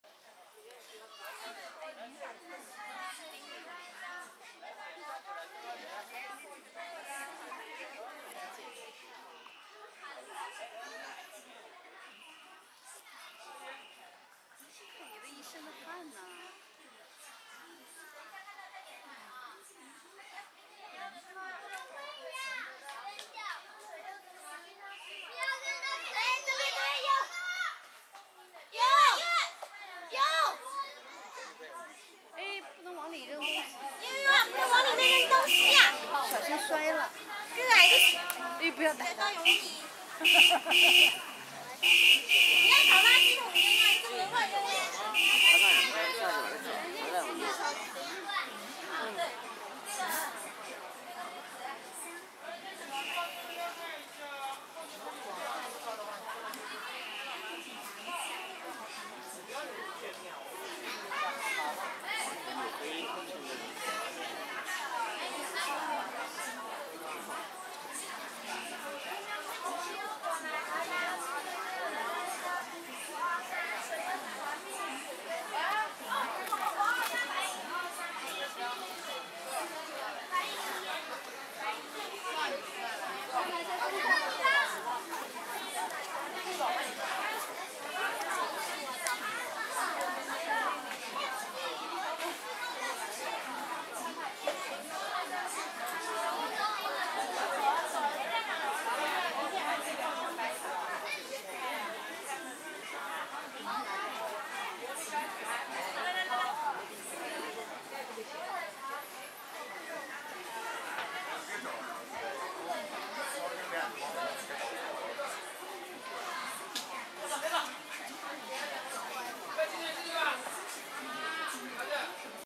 [2160p]記錄傍晚時分的西塘古鎮 In Record of XiTang Ancient Town in the Evening
Aug.16th 2017 18:14,Shooting at Old Theatre of Xitang Ancient Town,JiaShan Country,Zhejiang Province.
Aug.16th 2017 18:14,拍攝於浙江省嘉善縣西塘古鎮舊戲台。
Shooting Device:Sony Alpha 99 Mark II(ILCA-99M2)
拍攝設備:Sony Alpha 99 Mark II(ILCA-99M2)